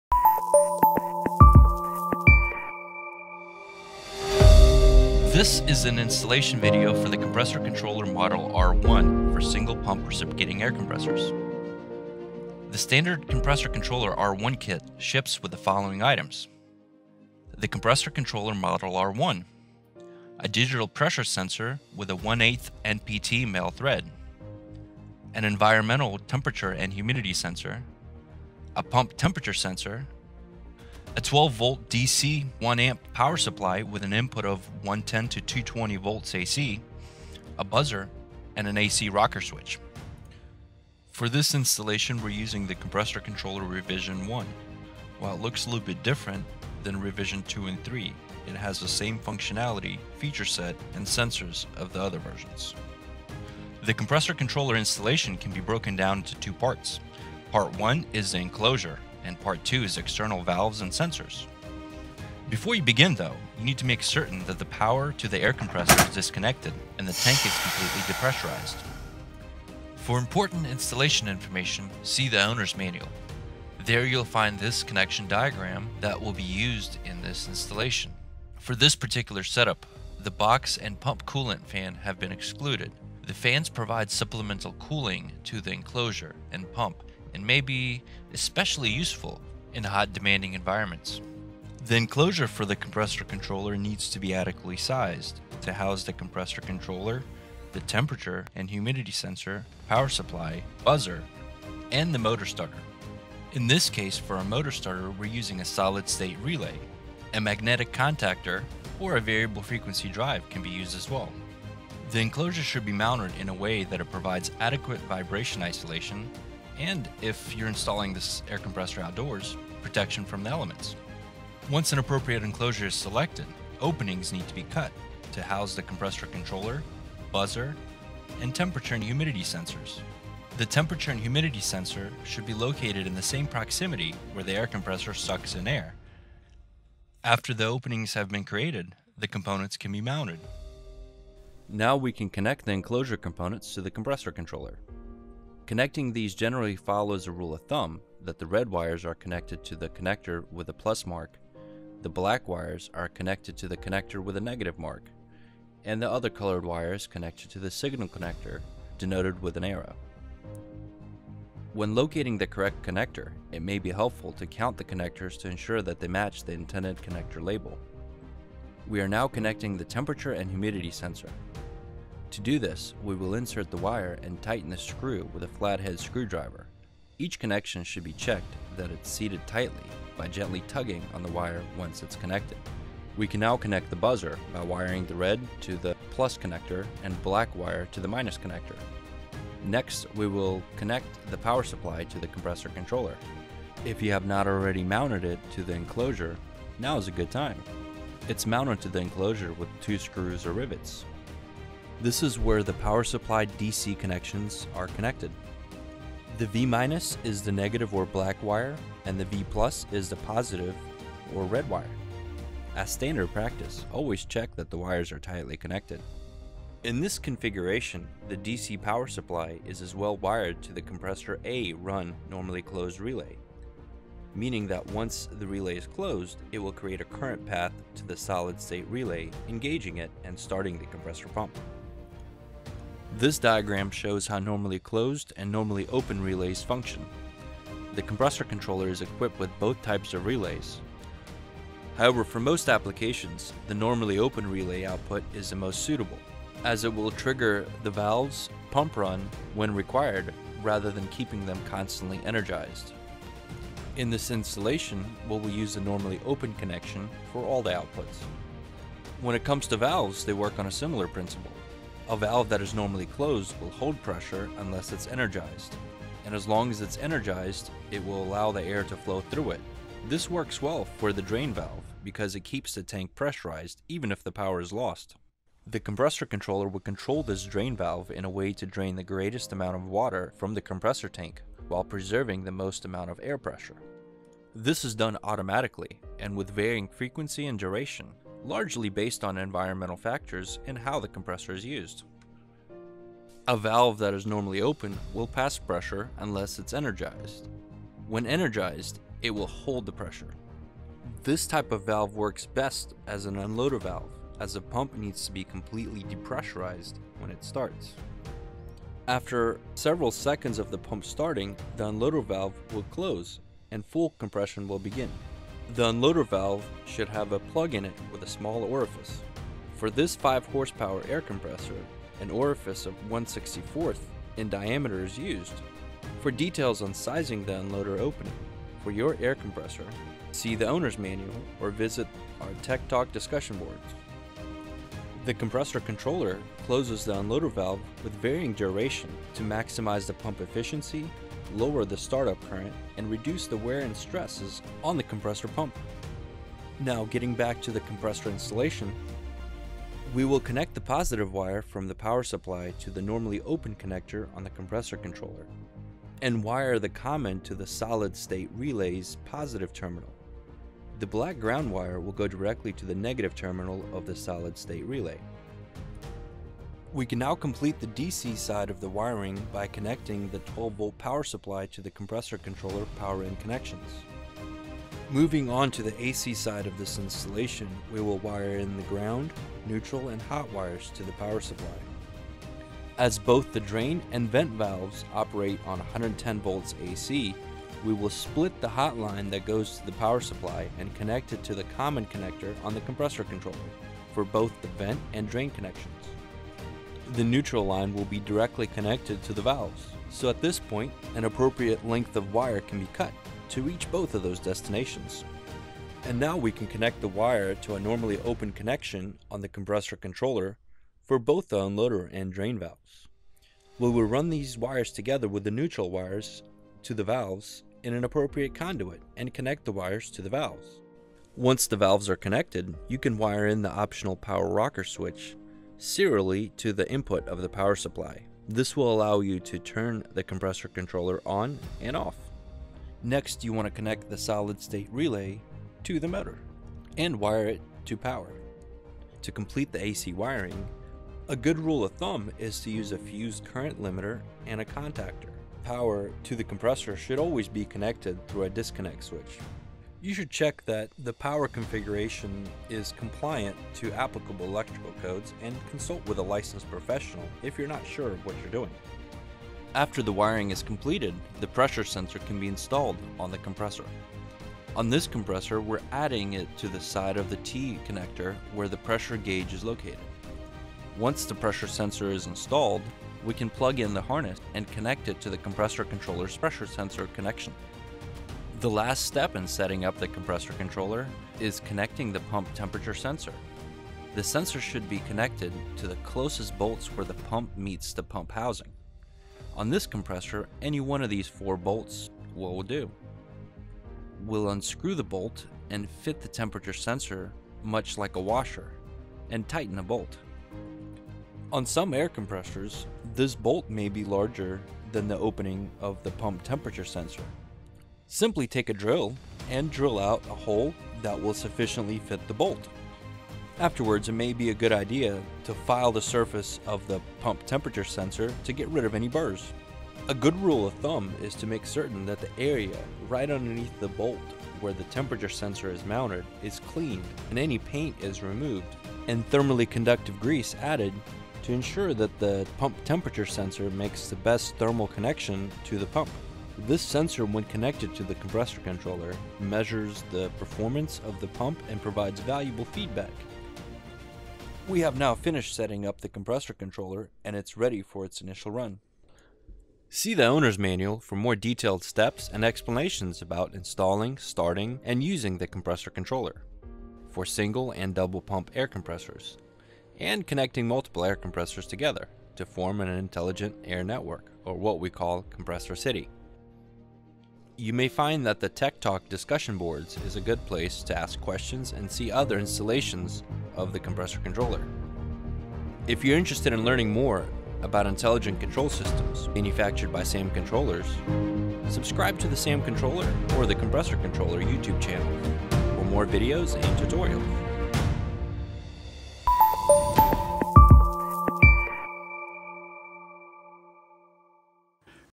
Compressor Controller R1 Compressor Installation Tutorial
This is a video tutorial for the installation of the compressor controller model R1 for single pump air compressors.